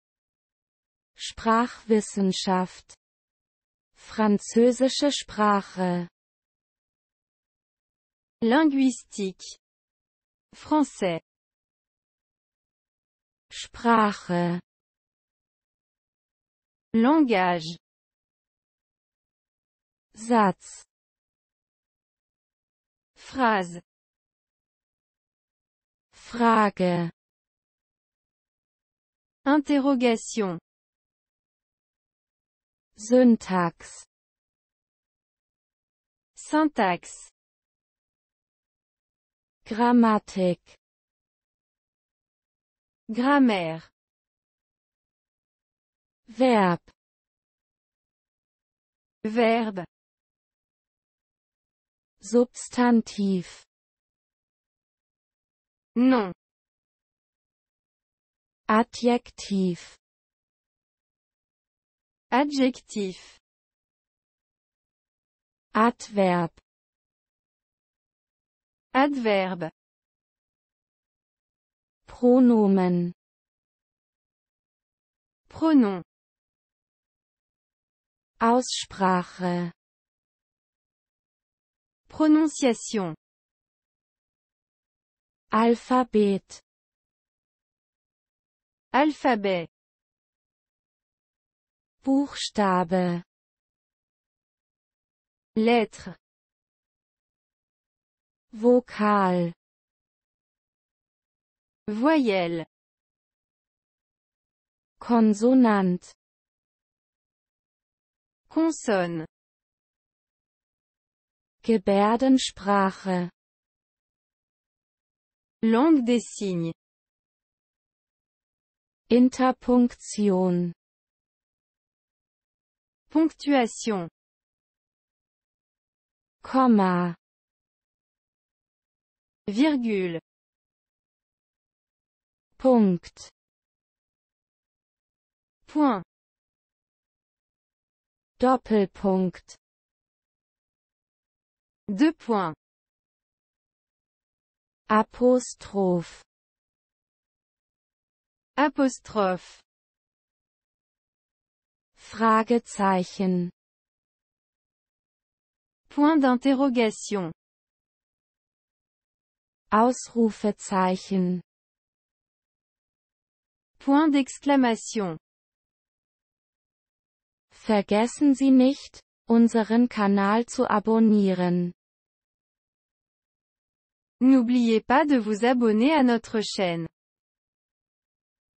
Sprachwissenschaft (Französische Sprache) (de-fr)
Fone TV
de-fr-36
Linguistique (Français)
Sprache - Langage
Satz - Phrase
Frage - Interrogation
Syntax - Syntaxe
Grammatik - Grammaire
Verb - Verbe
Substantiv - Nom
Adjektiv - Adjectif
Adverb - Adverbe
Pronomen - Pronom
Aussprache - Prononciation
Alphabet - Alphabet
Buchstabe - Lettre
Vokal - Voyelle
Konsonant - Consonne
Gebärdensprache - Langue des signes
Interpunktion - Ponctuation
Komma - Virgule
Punkt - Point
Doppelpunkt - Deux-points
Apostroph - Apostrophe
Fragezeichen - Point d'interrogation
Ausrufezeichen - Point d'exclamation